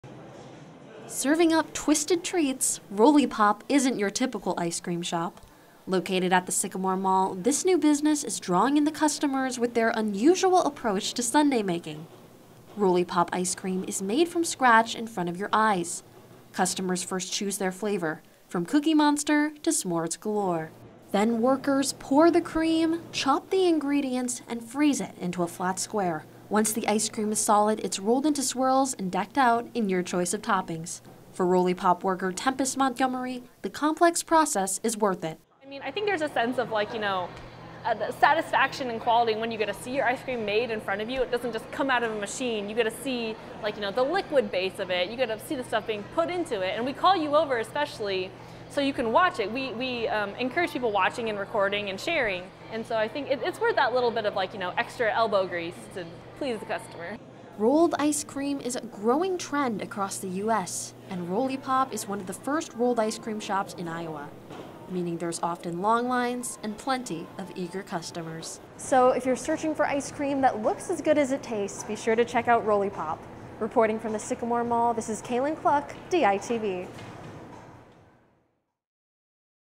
Rollipop Ice Cream
Rollipop in Iowa City is one of the first rolled ice cream shops in the state.